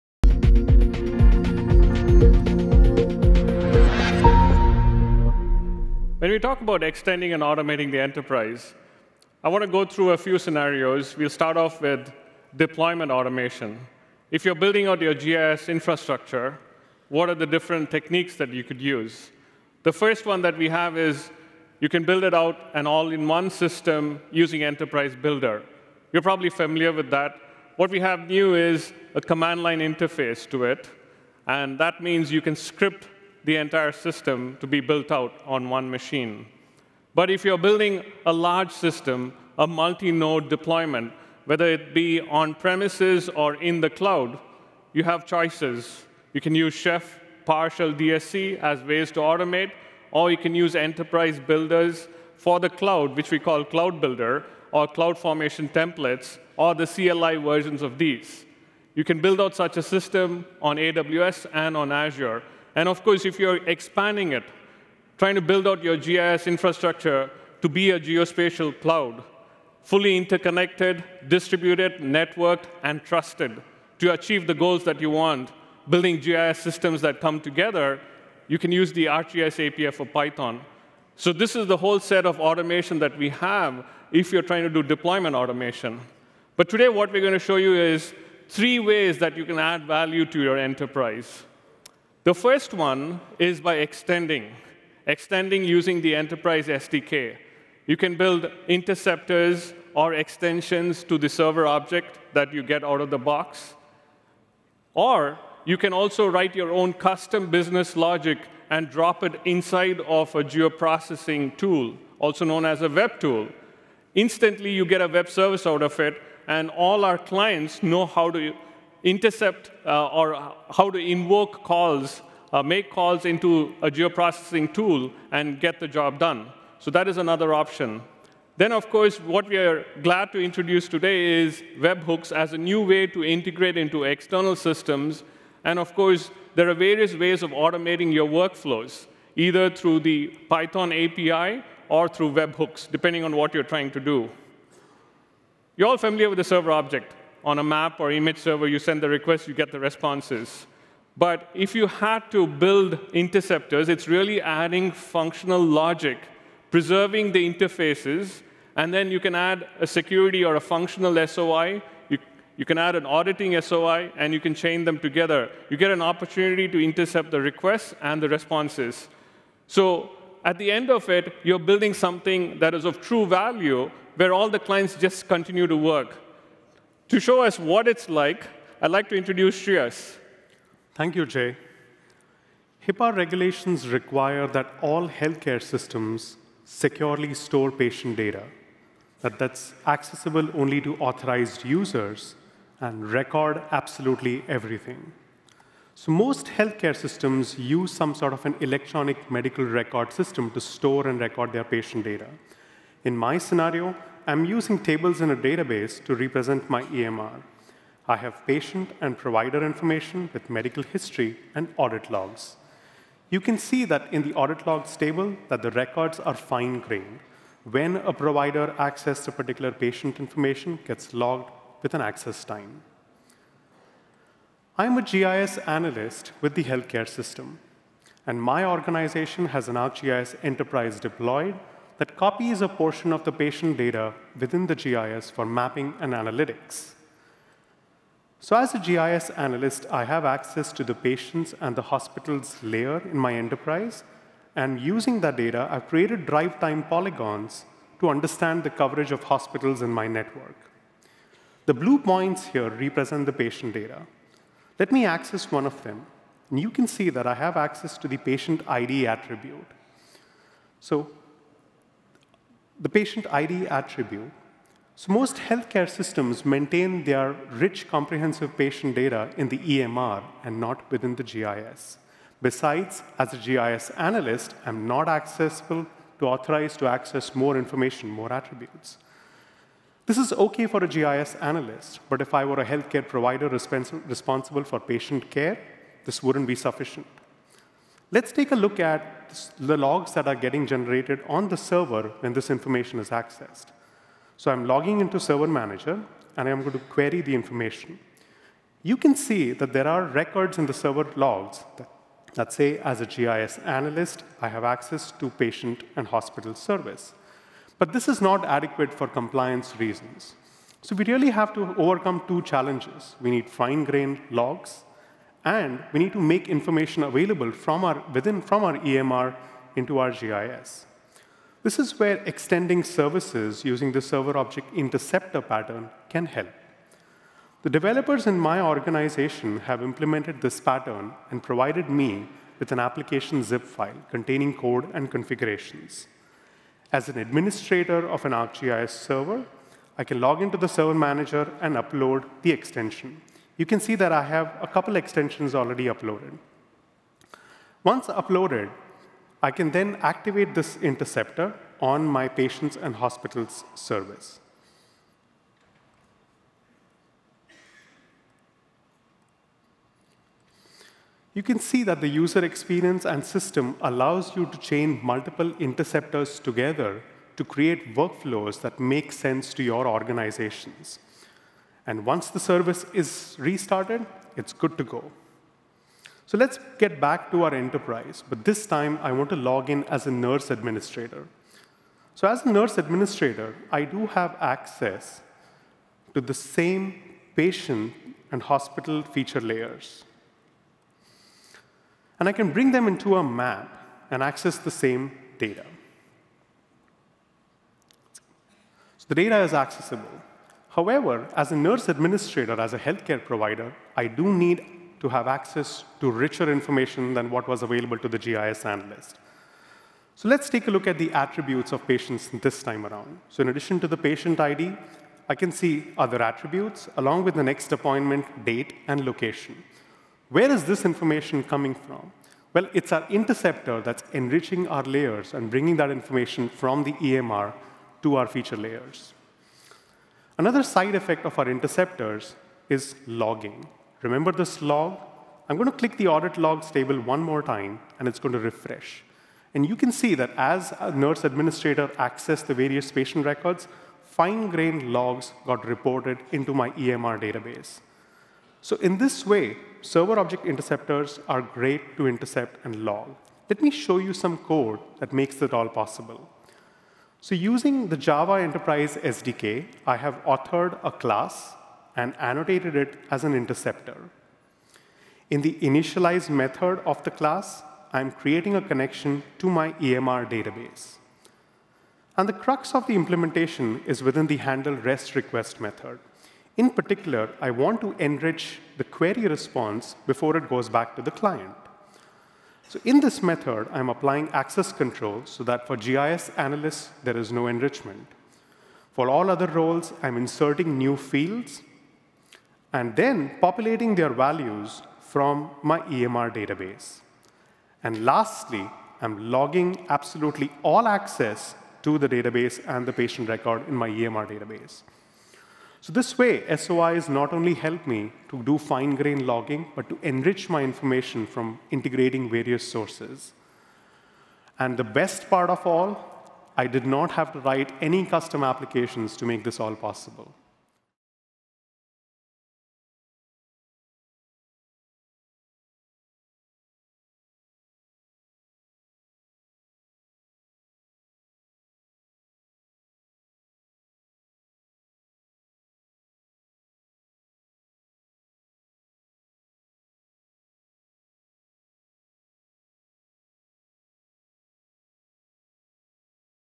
Server Object Interceptors
Lead Developer, Shreyas Shinde, demonstrates how to create a Server Object Interceptor (SOI) in order to include auditing, logging, and security capabilities around map data services.